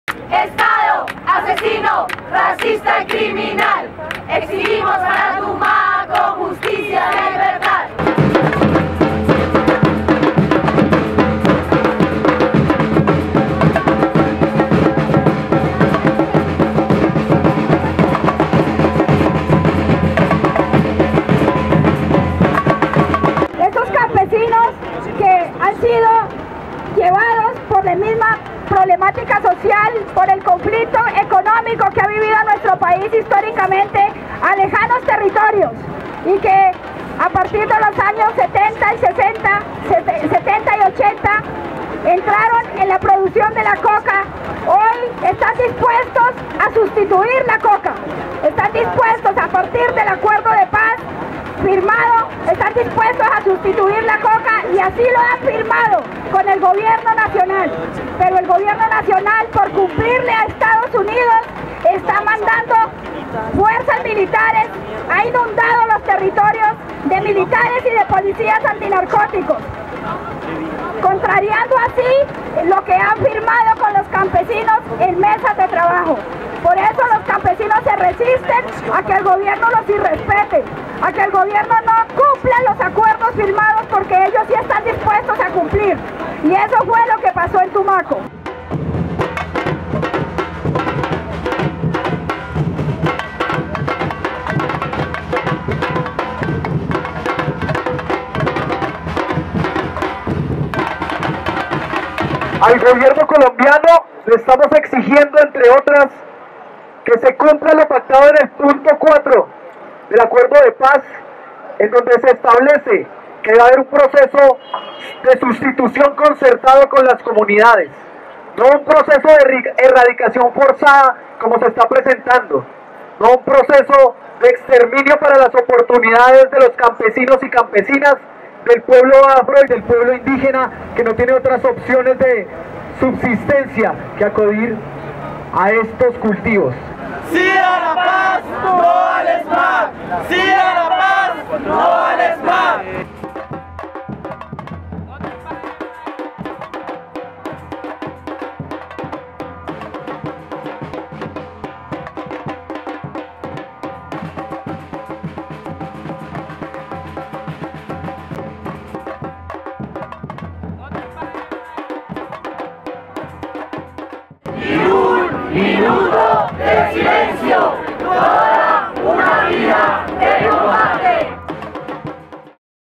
"This is what happened in Tumaco"
Social movements held a candle-lit vigil in Bogota Monday October 9 for the victims of the Tumaco massacre.

On October 5, Colombian security forces opened fire on coca farmers protesting attempts to forcefully eradicate their crops. At least 6 farmers were killed and 20 more injured. Witnesses say the death count will surely rise.

Colombian social movements have called nation-wide protests in response.